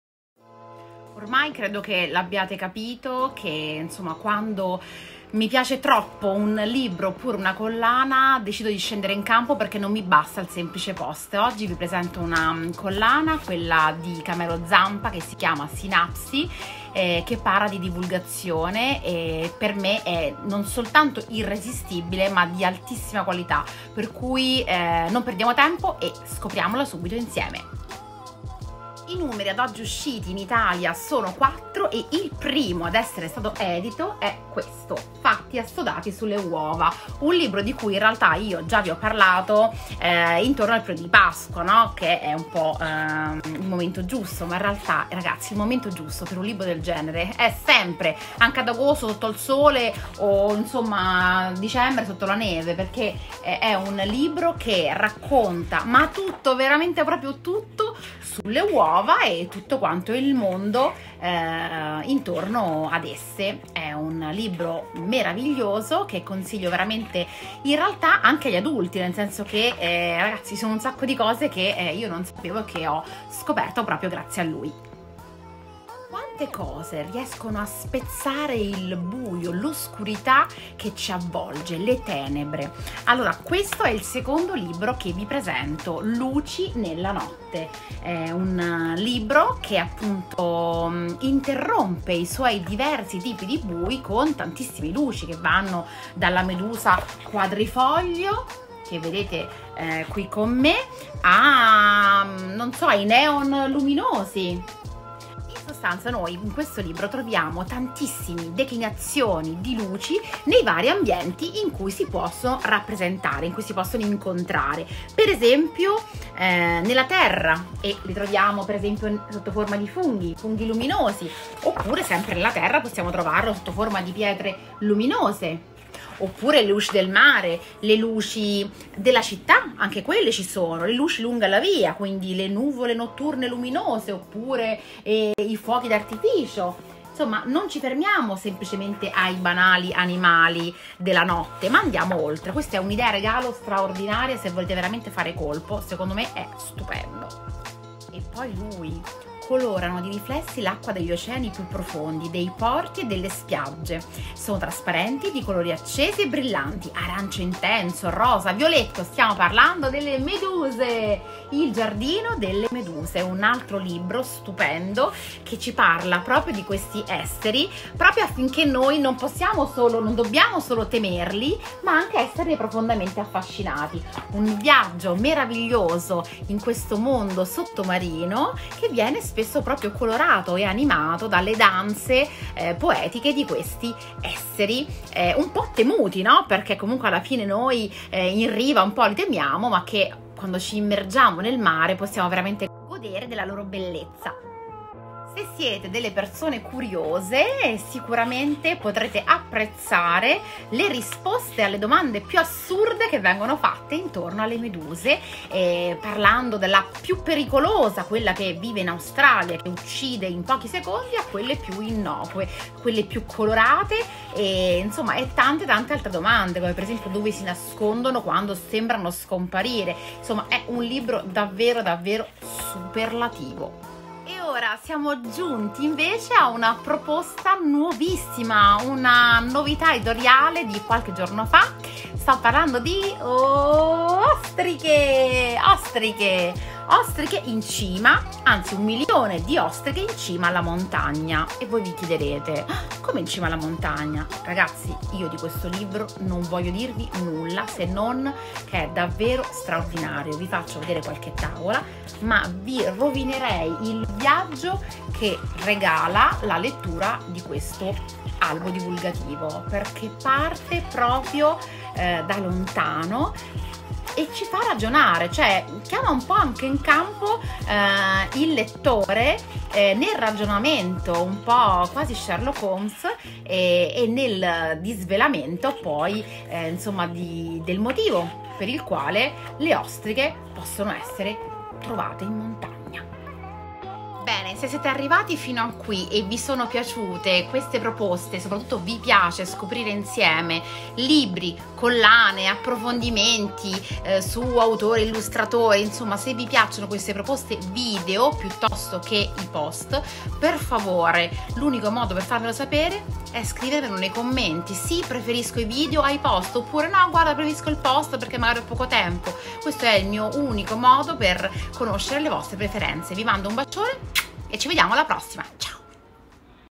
Libri di divulgazione: Una collana straordinaria!
Oggi vi presento Sinapsi, la collana di divulgazione di Camelozampa con quattro proposte irresistibili!!